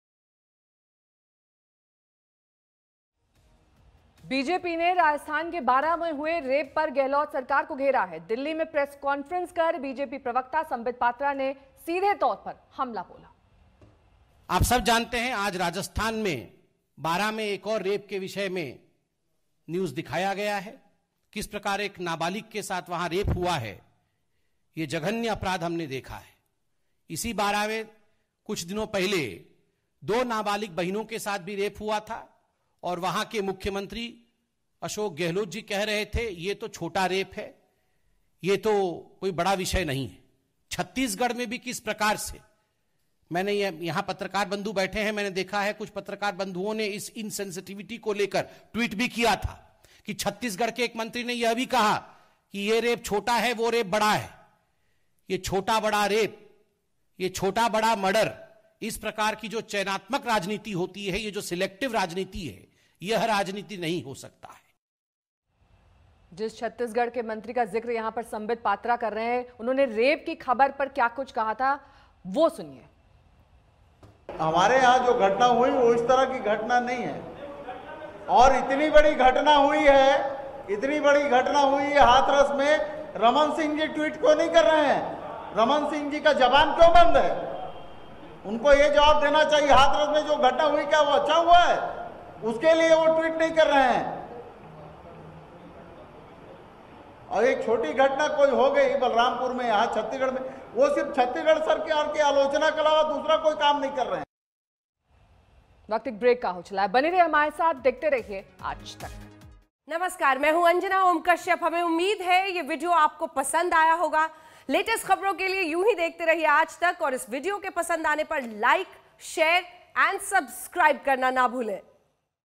Rape की घटनाओं को लेकर Sambit Patra का Congress पर हमला, बोले- सेलेक्टिव राजनीति नहीं होनी चाहिए
Rajasthan और Chattishgarh में हुई रेप की घटनाओं को लेकर कांग्रेस की ओर से की गई बयानबाजी को लेकर Sambit Patra ने हमला बोला है. उन्होंने कहा कि छत्तीसगढ़ के मंत्री कह रहे हैं कि हमारे यहा जो रेप हुआ वो छोटा था लेकिन जो हाथरस में हुआ वो ठीक नहीं हुआ. इस तरह की सेलेक्टिव राजनीति नहीं होनी चाहिए.